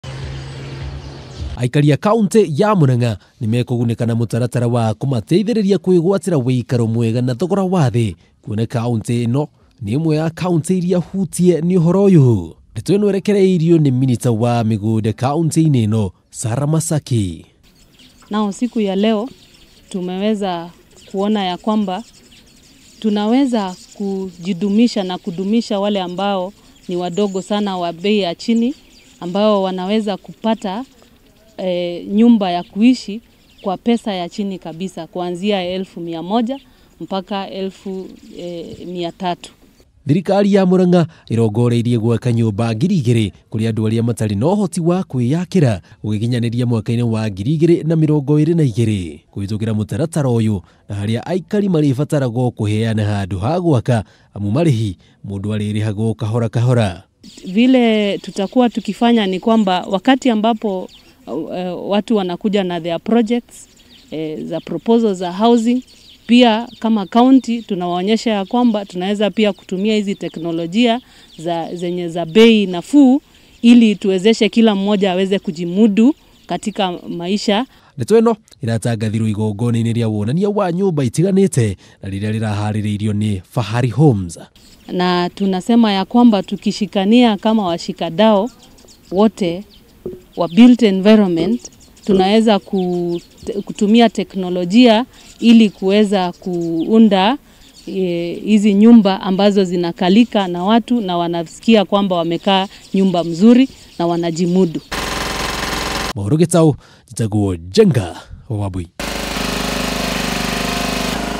Aikari a Murang'a kugunika na mutaratara wa gwakirwo nyumba cia thogora wa thi
Thirikari ya kaunti ya Murang'a niiruguritie mutaratara wa guteithiriria aikari kwigwatira wuikaro na njira ya raithi, ita njira imwe ya kuhingia kimwe kia itugi inya iria mutongoria wa bururi eriire Wanjiku.
Ndeto ino yaniriirwo ni minister wa migunda kaunti-ini ya Murang'a Sarah Masaki igongona-ini ria wonania wa nyumba itiganite na riria rirahariirio ni Fahari Homes.